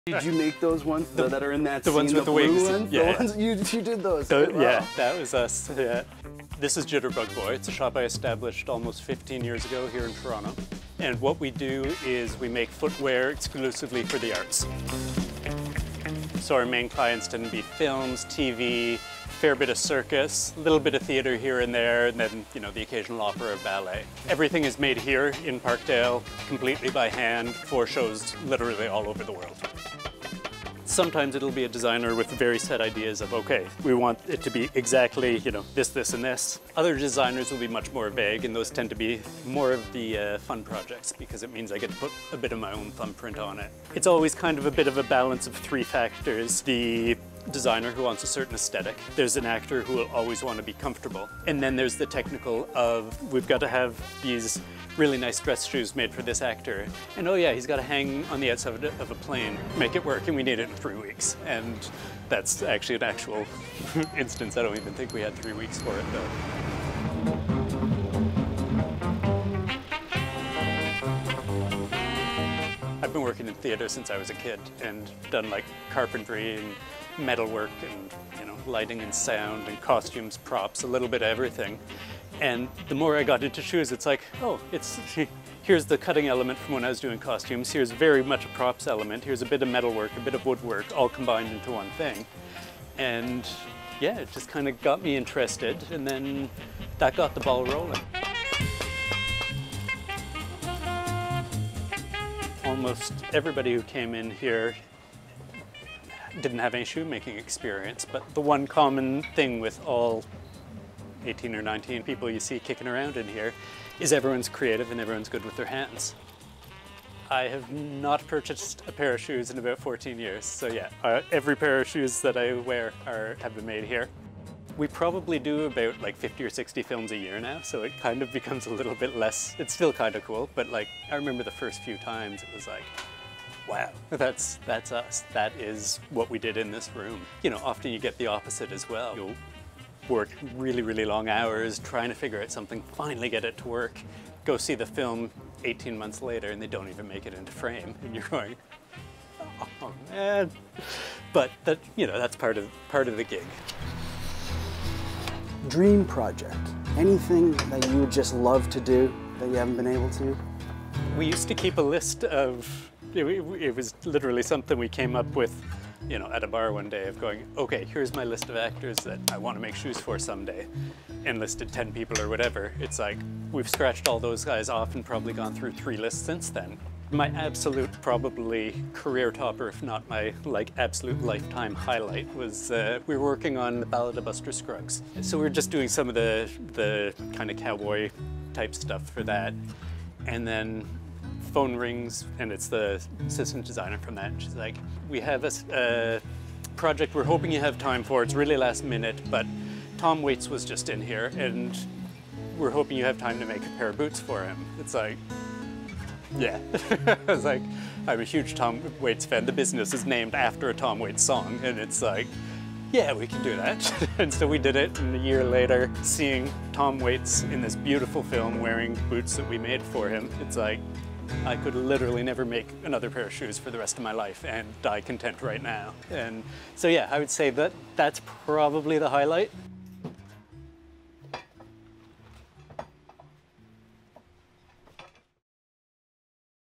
This Toronto shoemaker crafts footwear for Hollywood
Toronto shoemaker Jeff Churchill and his team at Jitterbug Boy have crafted footwear for films like Rocketman, Jojo Rabbit and Mary Poppins. CBC's Lucas Powers takes us inside the workshop.